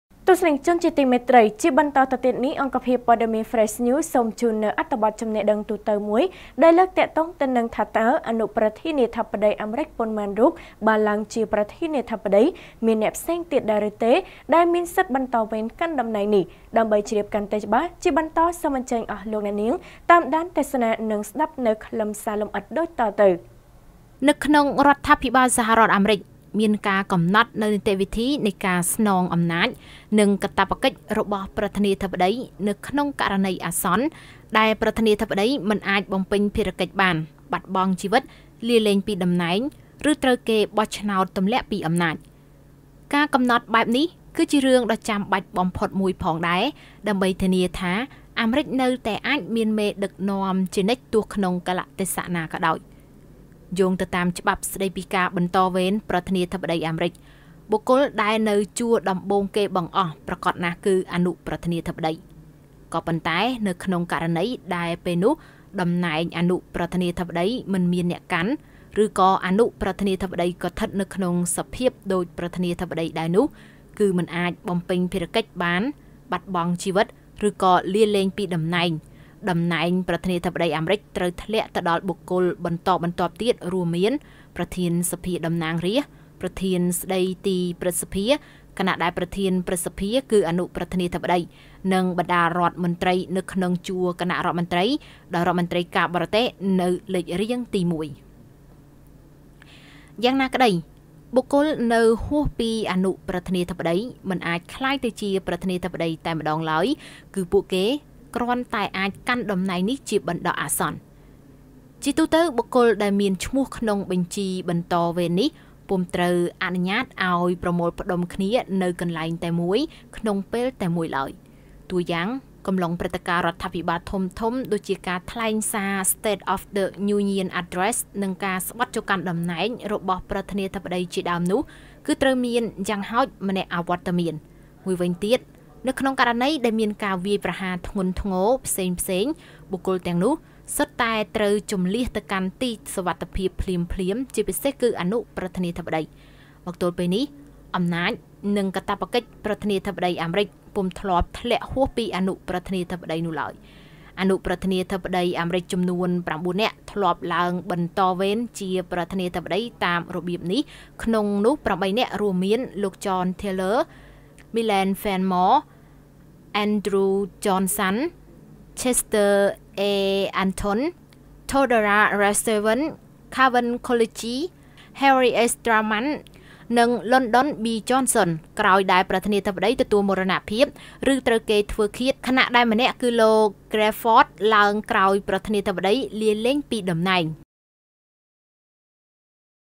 ចំណេះដឹងទូទៅ៖ អនុប្រធានាធិបតីអាមេរិកប៉ុន្មានរូបបានឡើងជាប្រធានាធិបតី, មានអ្នកផ្សេងទៀតដែរទេ...
ចំណេះដឹងទូទៅ៖ អនុប្រធានាធិបតីអាមេរិកប៉ុន្មានរូបបានឡើងជាប្រធានាធិបតី, មានអ្នកផ្សេងទៀតដែរទេ ដែលមានសិទ្ធិបន្តវេនកាន់តំណែងនេះ?